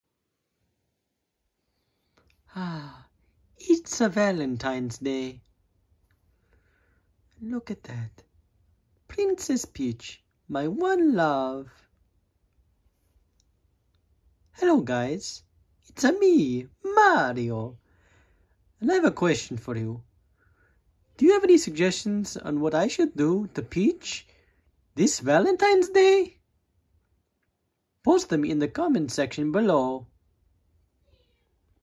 Mario Wonders About His Valentine
It’s Valentine’s Day and Mario wants to know what he wants to do for Princess Peach on Valentine’s Day.